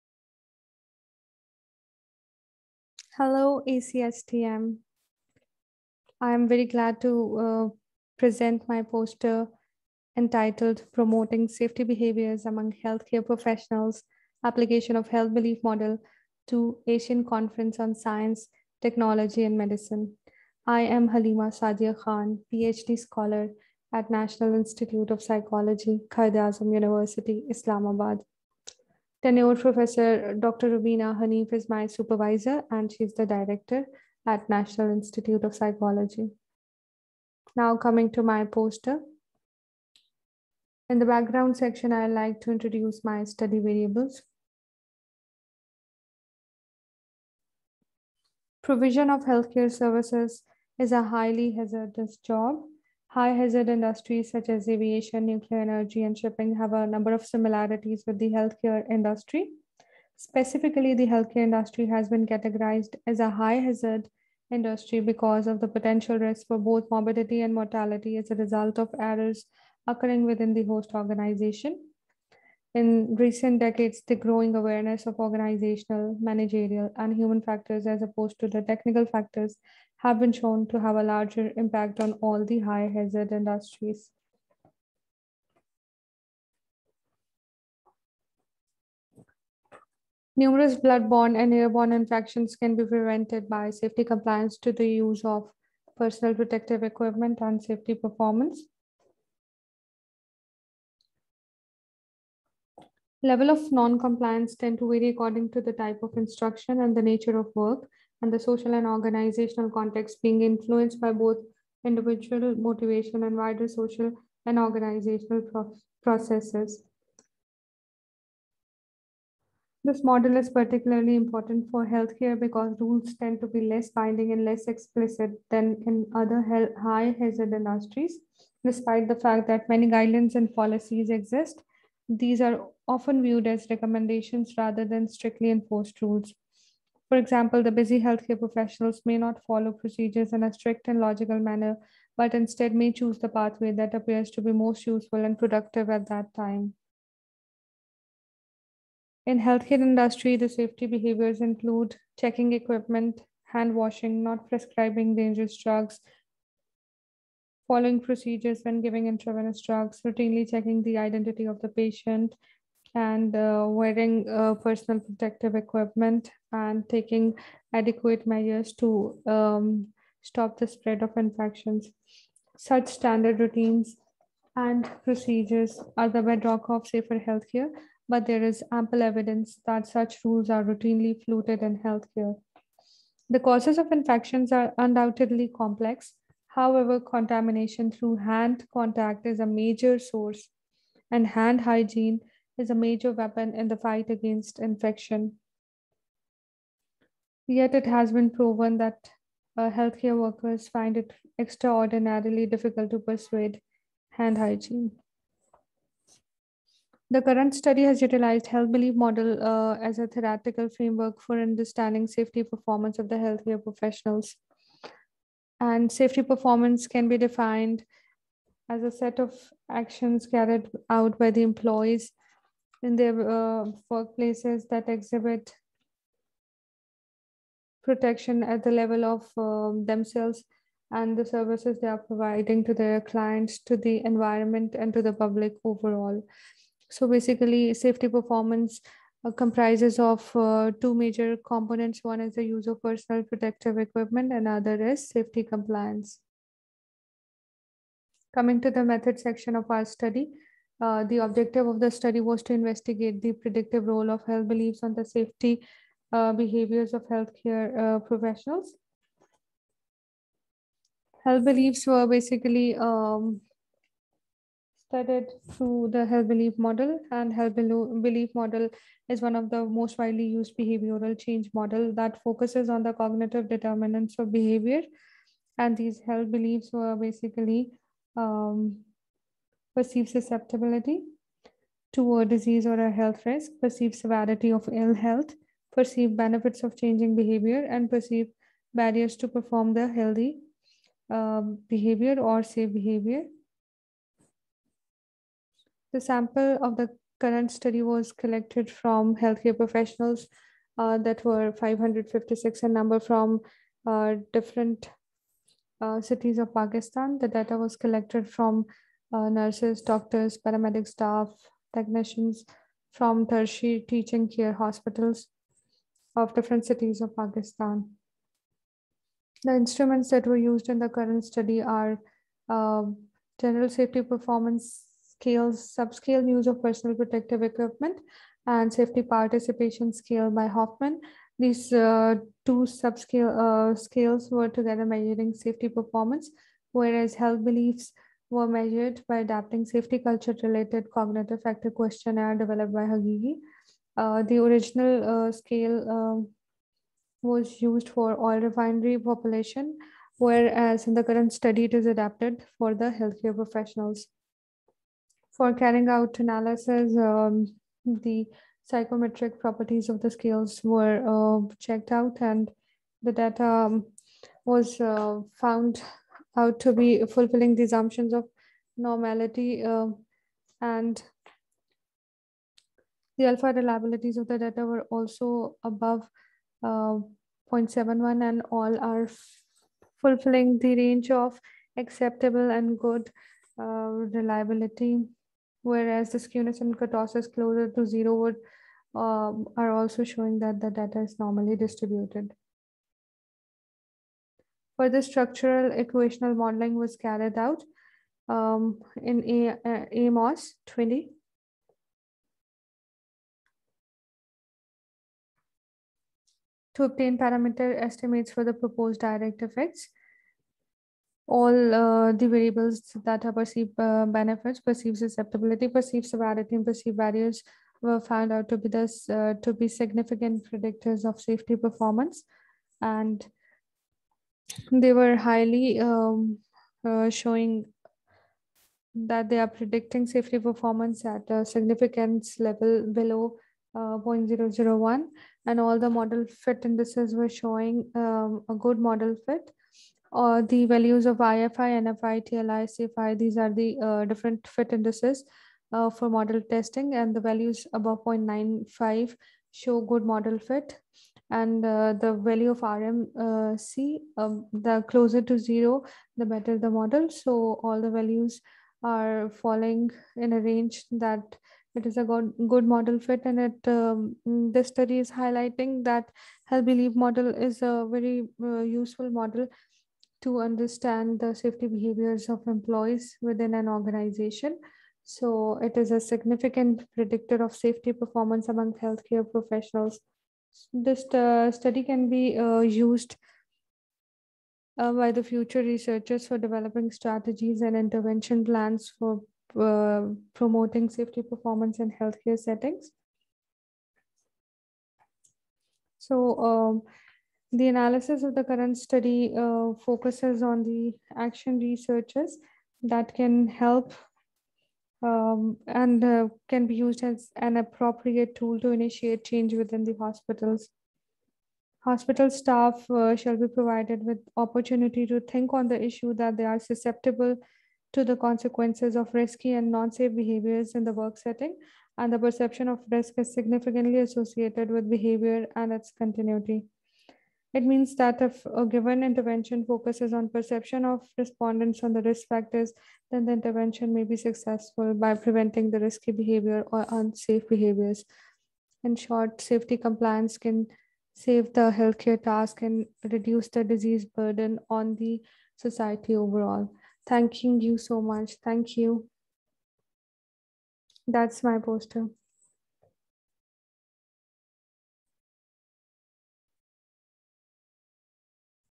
Haleema Saadia Khan | Poster Presenter | NIP, Quaid-i-Azam University Islamabad, Pakistan
Title of Presentation:
Promoting Safety Behaviors Among Health Care Professionals: Application of Health Belief Model

Speaker Details:
Haleema Saadia Khan
National Institute of Psychology, Quaid-i-Azam University Islamabad, Pakistan